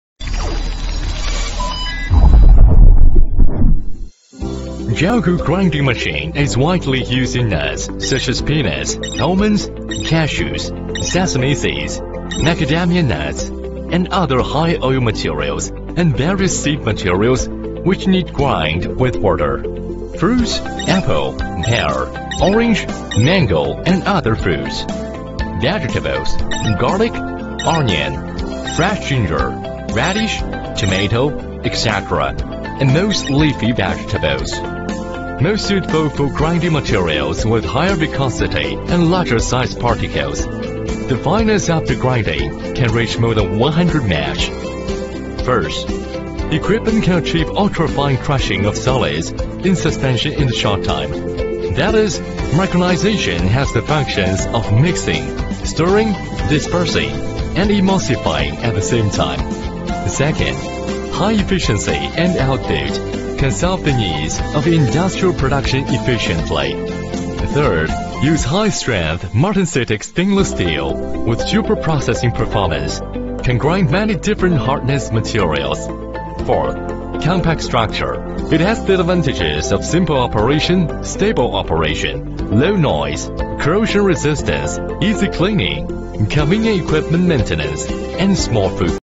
Industrial Vegetable and Fruit Grinding Machine/ Commercial Vegetable and Fruit Grinder
In the video, the automatic vegetable and fruit grinding machine can grind a great variety of vegetables and fruits, like red chili pepper, tomato, ginger, garlic, fruits and so on. Gelgoog can design and manufacture the grinding machines to meet customer needs.

Advantages of Veggie and Fruit Grinder
1. Multipurpose. It can be used to grind a great variety of vegetables and fruits with great efficiency.
2. Compact designs with small foot prints.
3. 304 stainless steel structure.
4. Easy to clean and maintain.